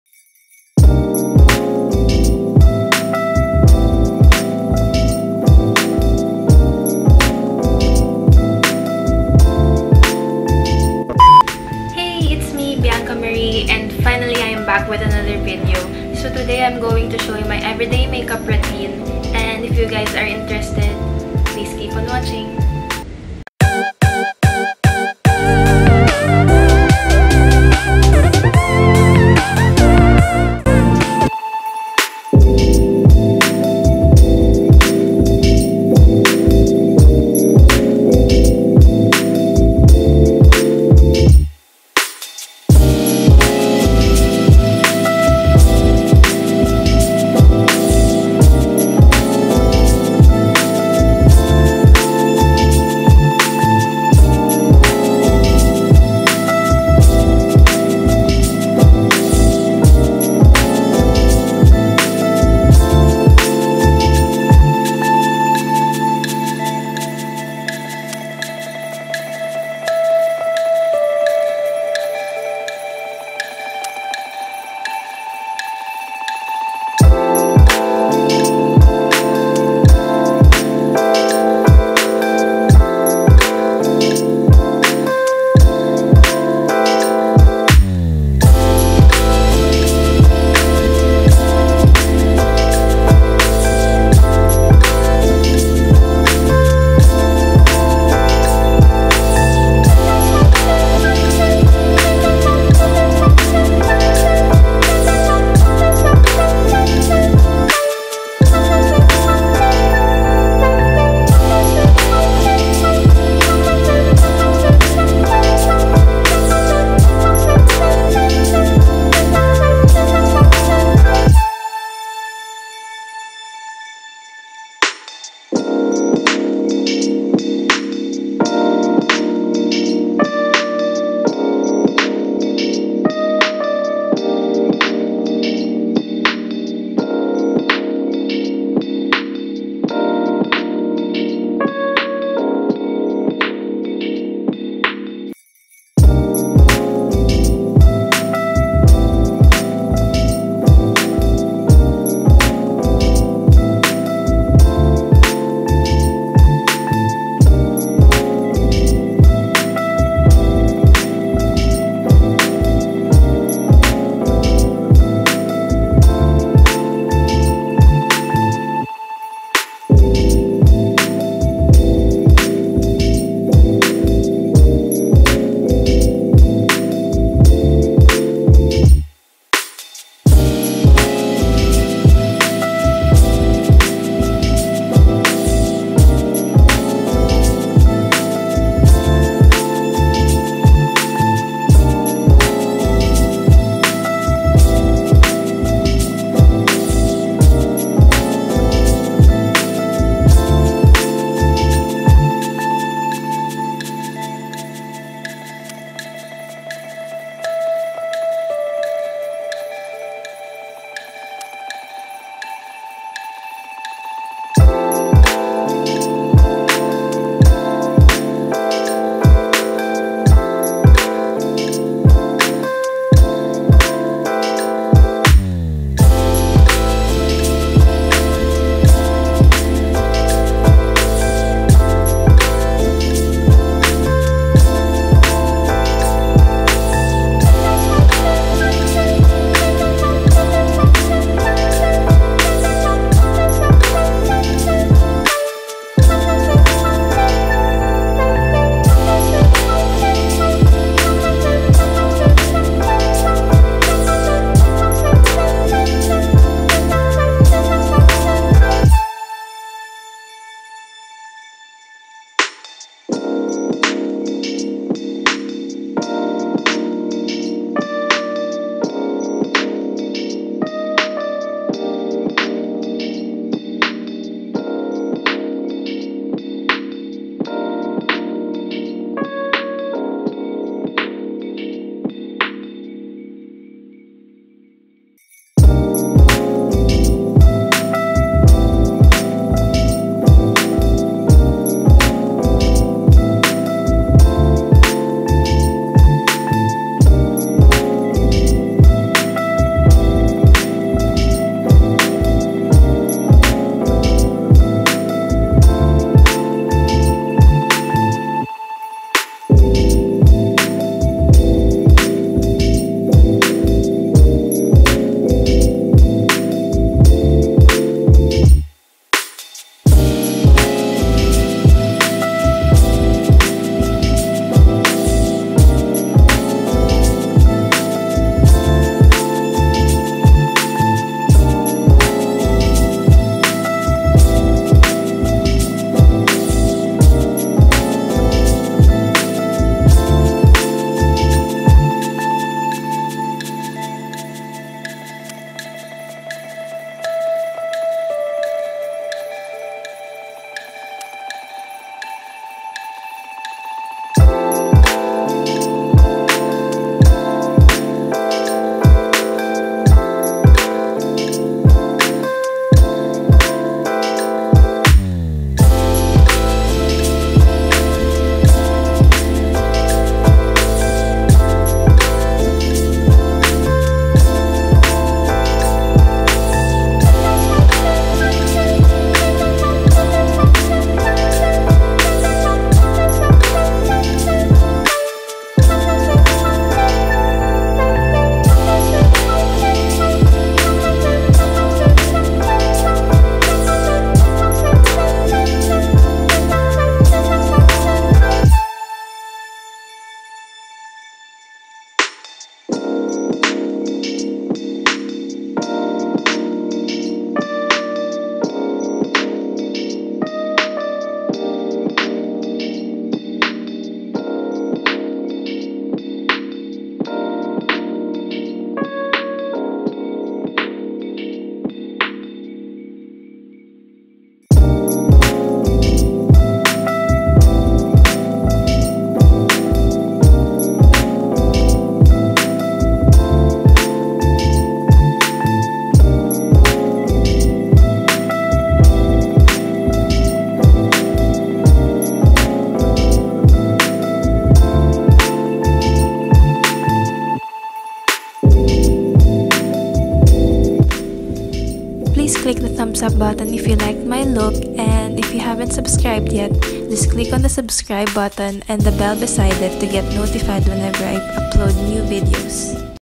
Everyday Makeup Routine (2019!) | Bianca Marie🌻
*Products used:
Mario Badescu Facial Spray
The Face Shop Designing Eyebrow Pencil
The Saem Tip Concealer
Colourette Coloursnap (Milk)
Maybelline Clearsmooth Powder
Snoe Flash On Mascara
Happy Skin x Love Marie (Skinny Dipping)
H&M Illumination Lustre Powder